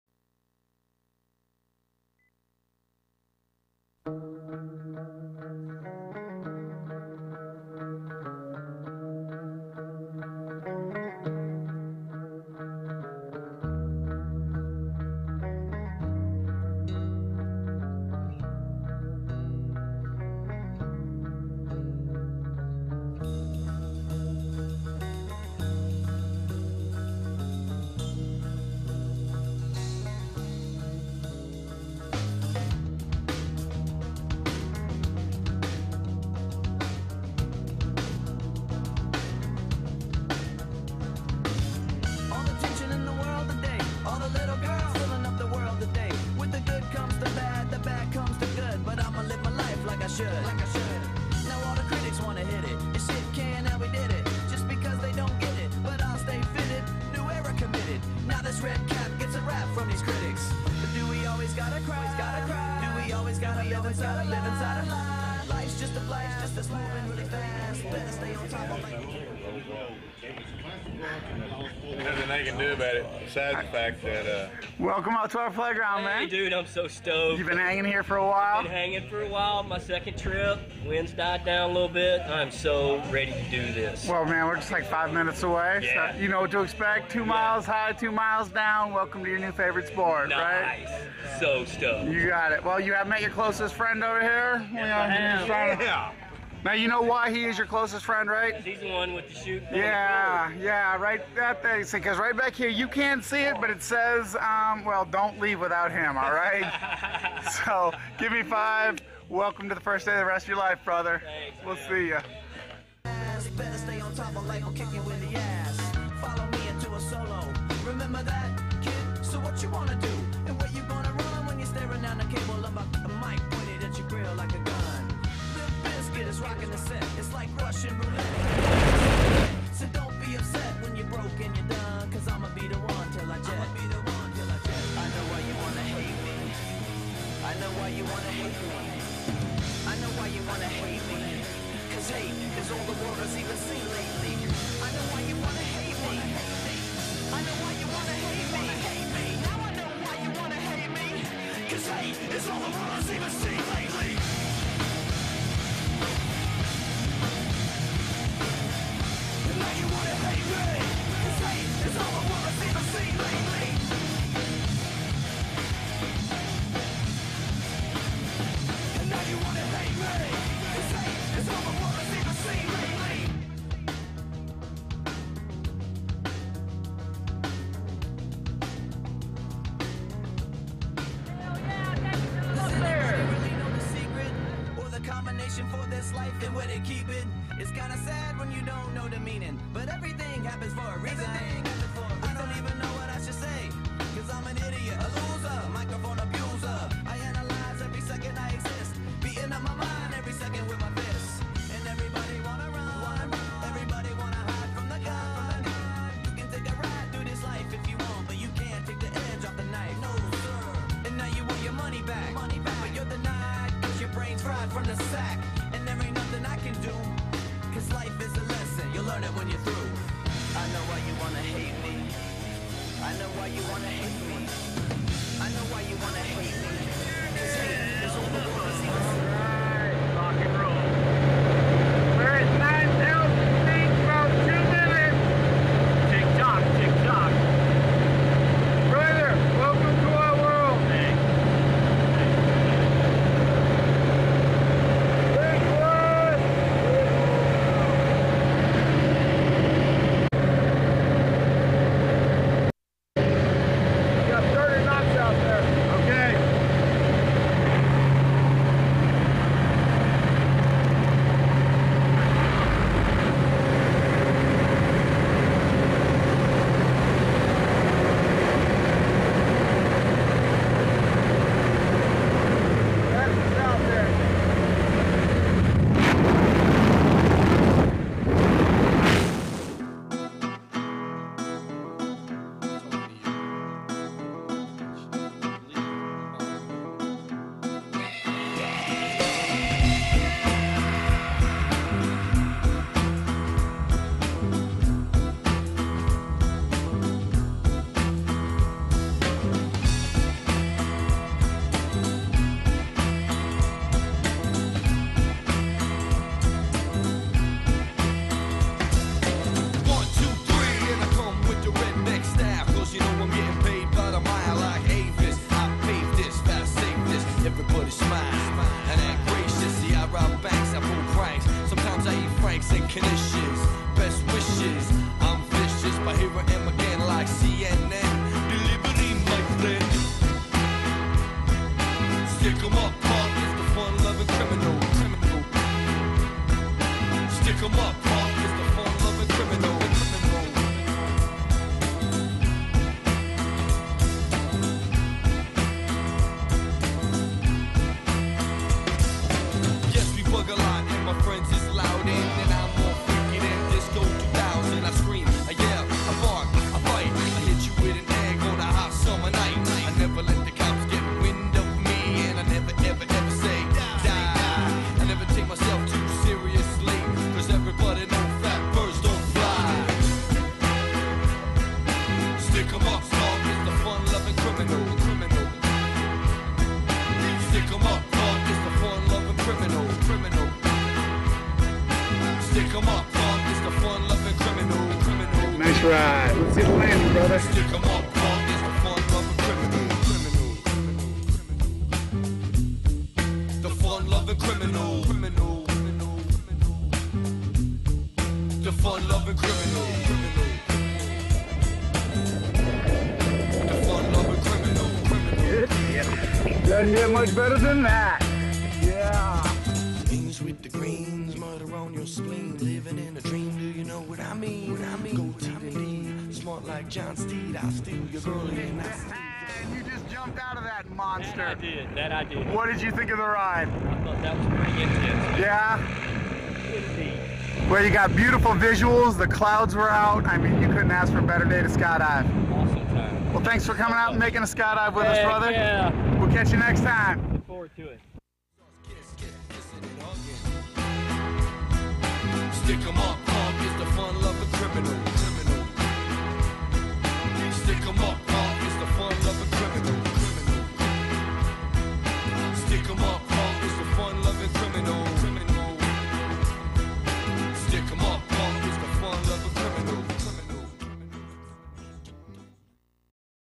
skydive.m4v
Cory's 1st skydive tandem jump in Texas